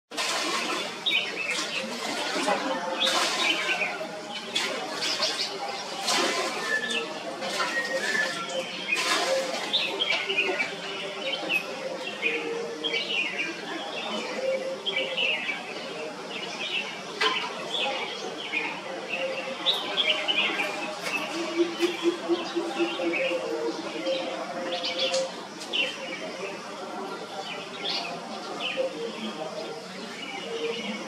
Andaman (brown) coucal
An Andaman Coucal foraging on a terrace near our house at Haddo, Andaman.